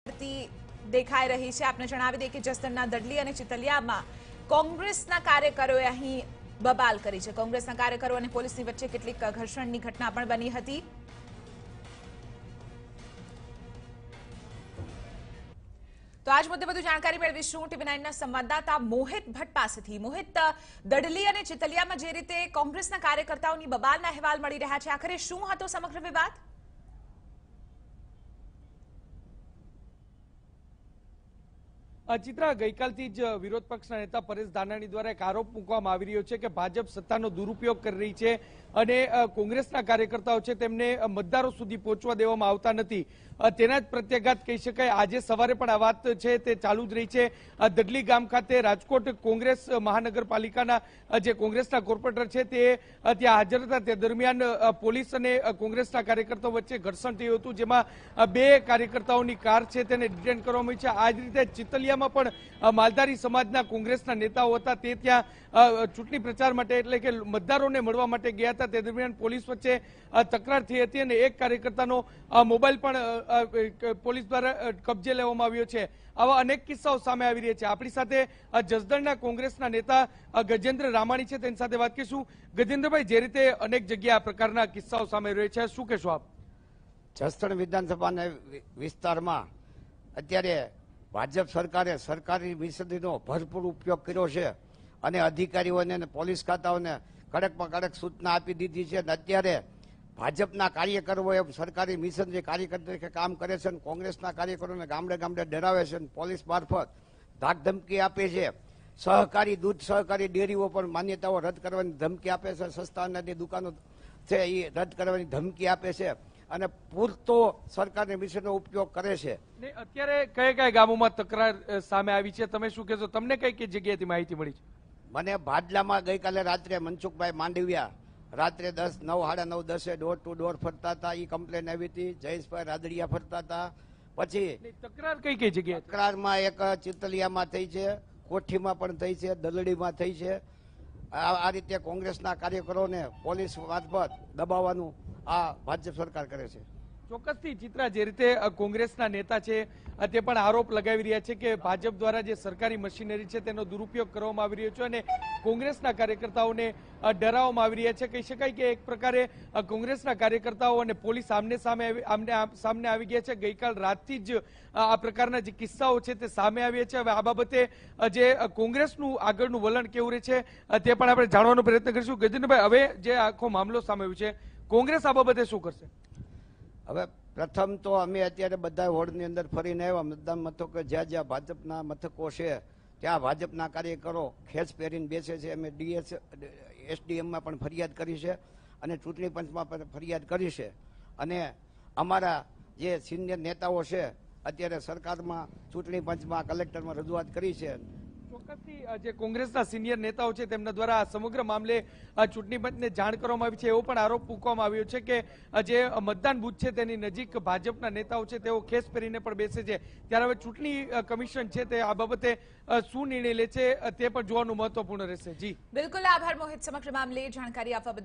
Jasdan by-poll 2018 : Clash breaks out congress workers and police at Dadli & Chitaliya - Tv9
Jasdan by-poll 2018 : Clash breaks out congress workers and police at Dadli & Chitaliya.